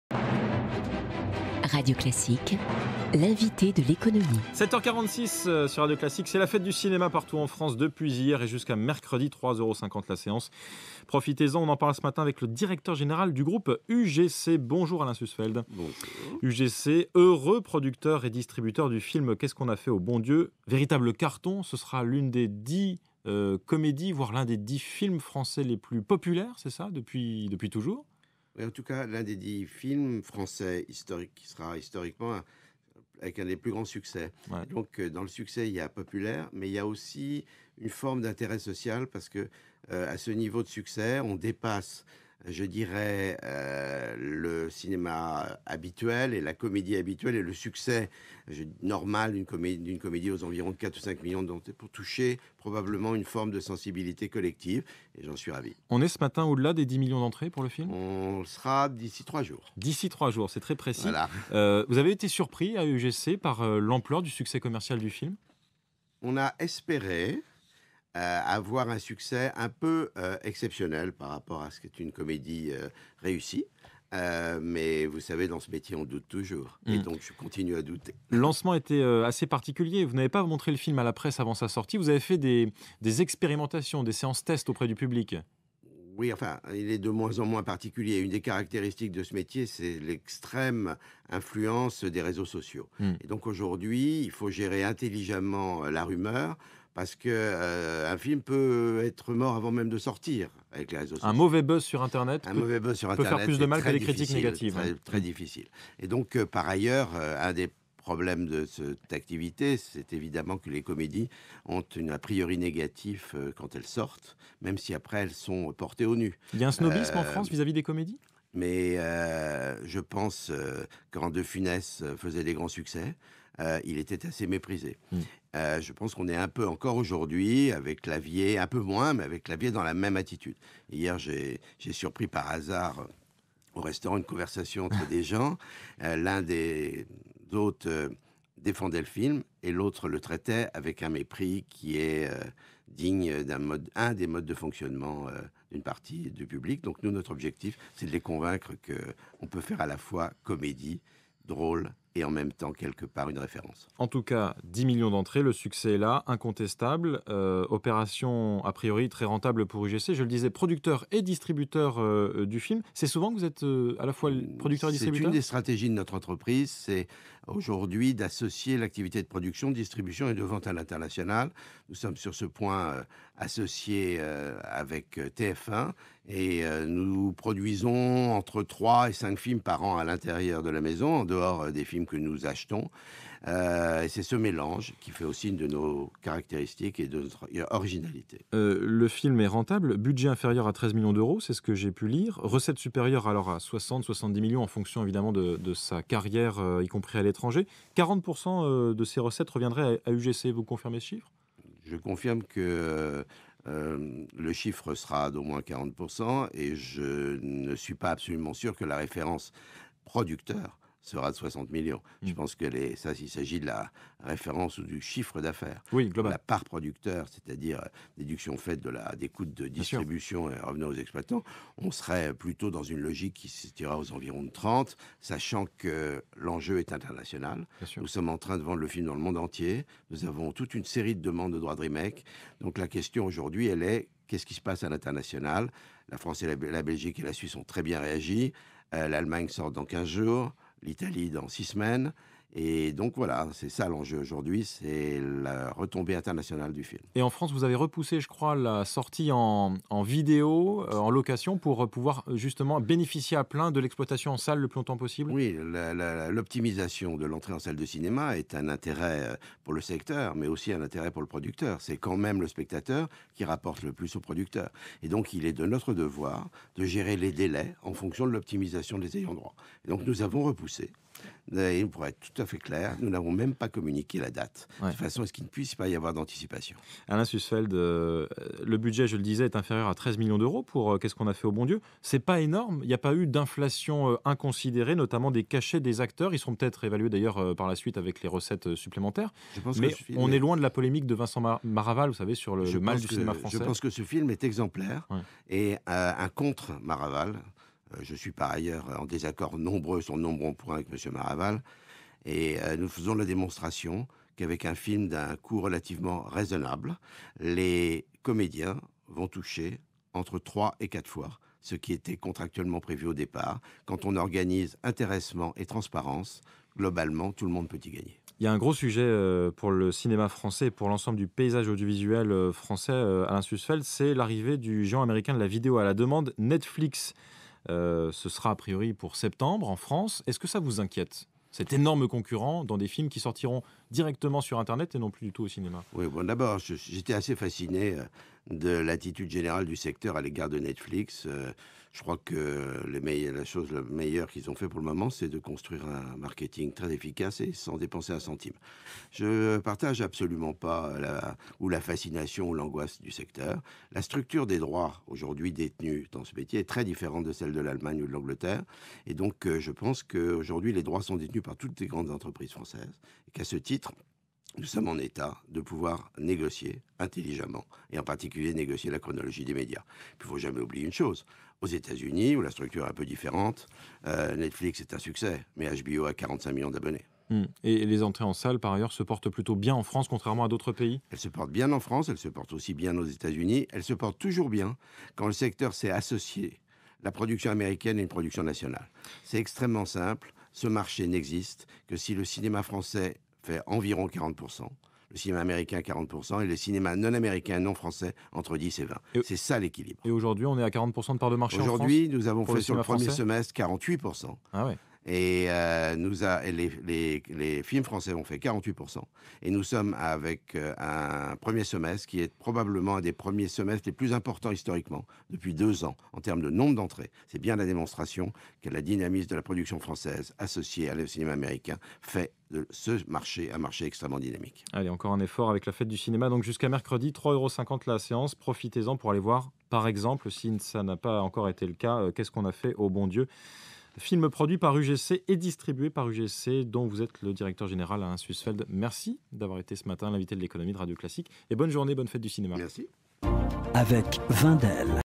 Alain Sussfeld, invité de l'économie
A 7h45, le Directeur Général d'UGC Alain Sussfeld est l'invité de Nicolas Pierron.